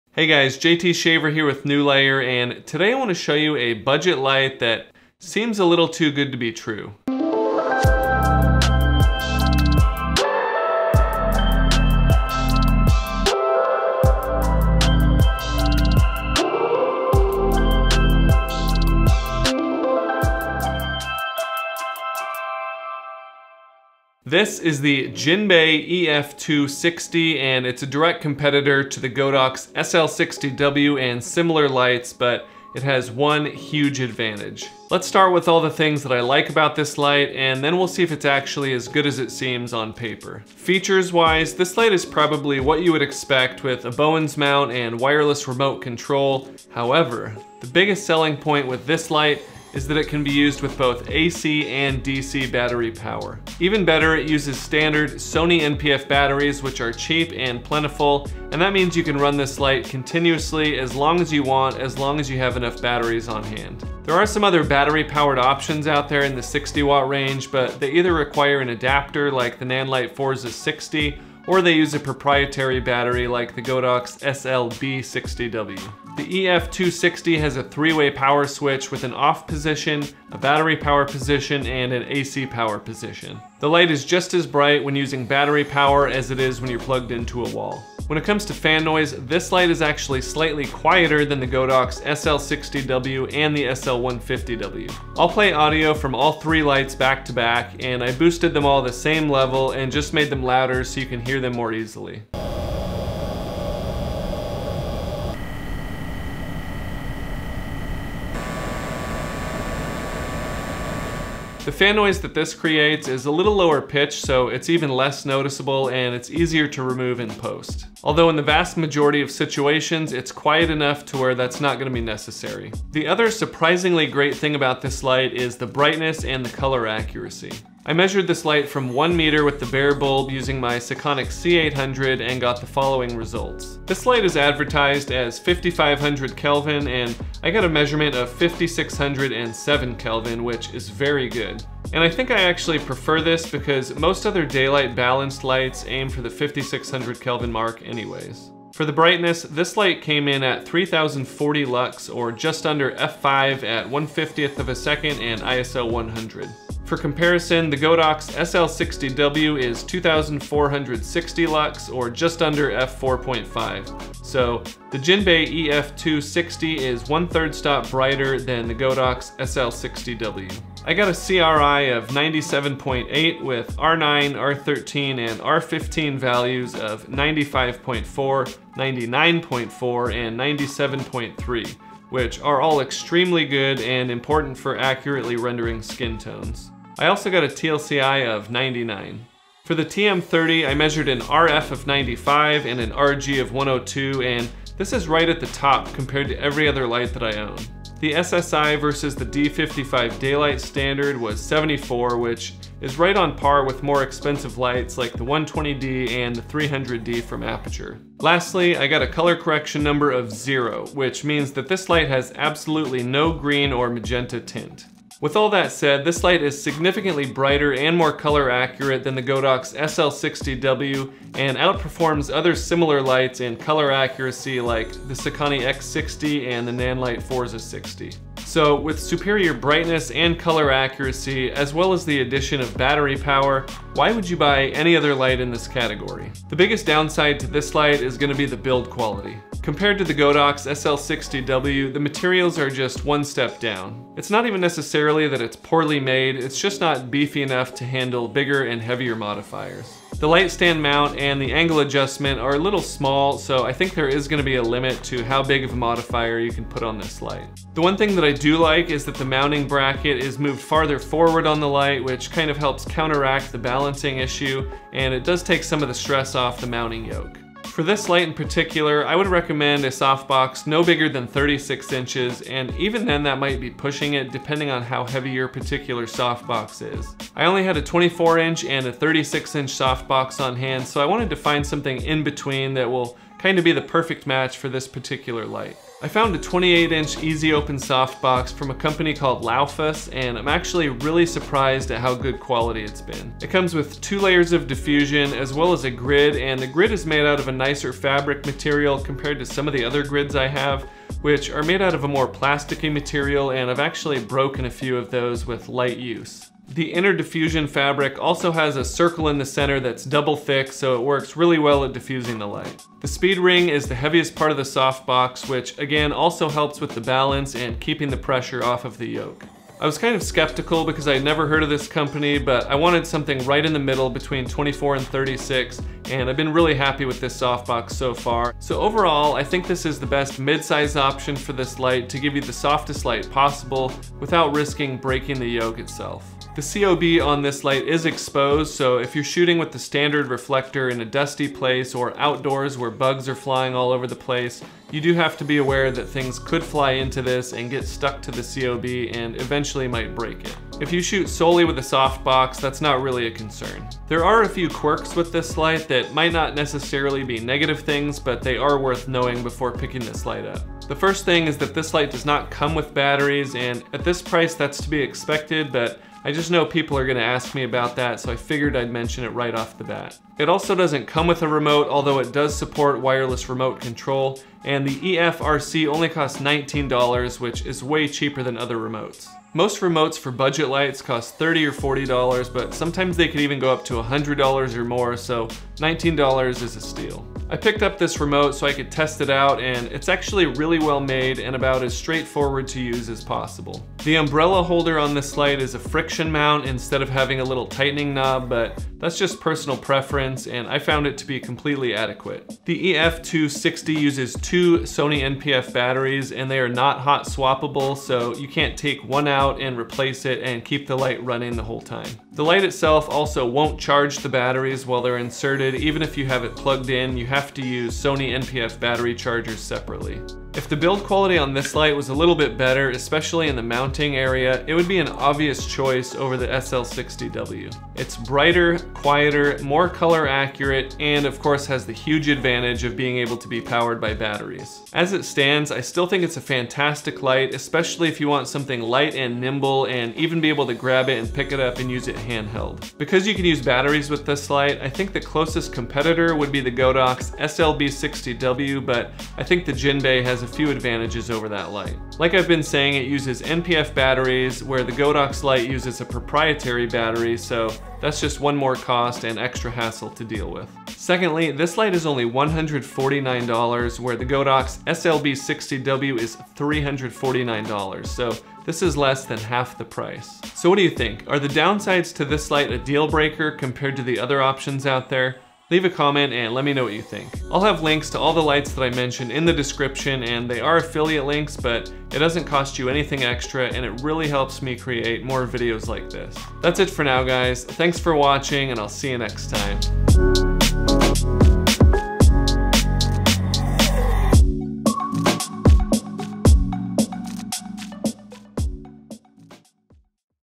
Battery-Powered Godox SL60W Killer? (Jinbei EF II 60 Review & Comparison)
In this video I review the Jinbei EF II 60 LED video light and compare it to other popular 60 watt lights like the Godox SL60W, Nanlite Forza 60, and Sokani X60.

The Jinbei EF II 60 is one of the only COB lights with built-in battery support, and the only light I know of that natively supports Sony NP-F batteries.

The Godox SLB60W is similar, but requires a proprietary battery, and costs more than twice as much. The Nanlite Forza 60 also has battery support, but requires a special adapter.

Surprisingly, the Jinbei EF II 60 is also brighter and more color accurate than the Godox SL60W and SLB60W, and renders colors better than the Nanlite Forza 60 and Sokani X60 as well.

So with superior brightness and color accuracy, along with native NP-F battery support, why would you choose something like the popular Godox SL60W instead of this alternative? There are a few downsides to this light, so watch the whole video to see if they might be a deal breaker for you.

Jinbei EF II 60
Jinbei EF-RC Remote
Laofas 27.5" Softbox

Godox SL60W
Godox SLB60W
Nanlite Forza 60
Sokani X60

0:00 - Product B-Roll
0:23 - Intro
0:34 - Positive Aspects
1:27 - Fan Noise Comparison
2:01 - Brightness/Color Accuracy
3:47 - Negative Aspects
4:26 - Recommended Softbox
5:53 - Quirks
6:42 - Battery Info
6:59 - Final Thoughts
7:40 - Price Comparison